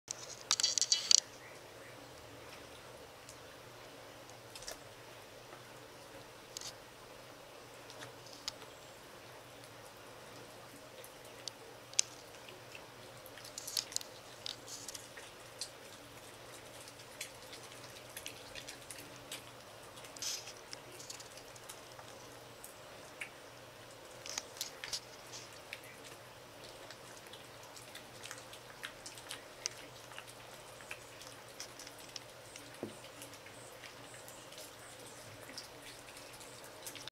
Lady and Cowby eating fish together
Lady and Cowby enjoying some fresh salmon